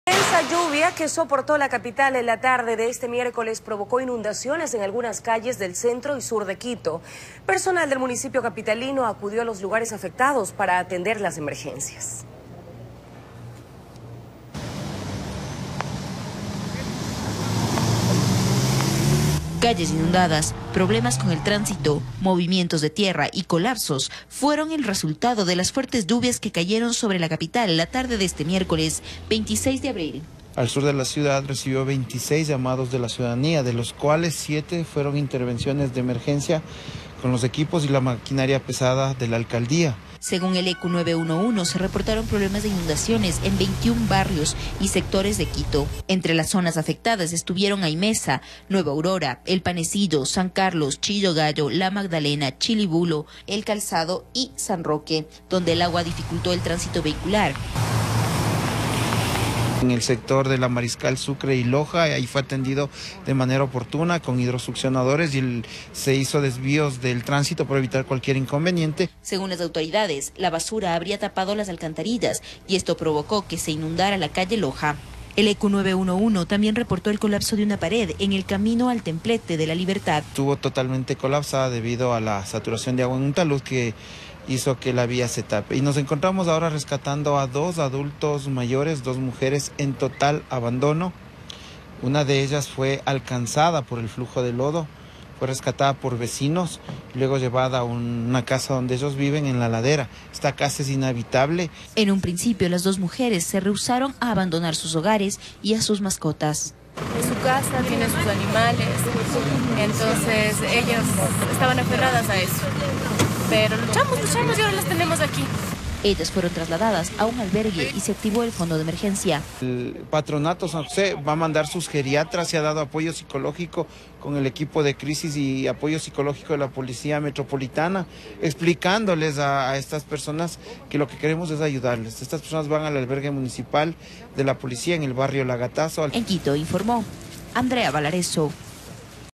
Intensa lluvia provocó inundaciones en Quito
Calles inundadas, problemas en el tránsito, movimientos de tierra, y colapsos, fueron los principales problemas por las fuertes lluvias. Hubo siete intervenciones de emergencia.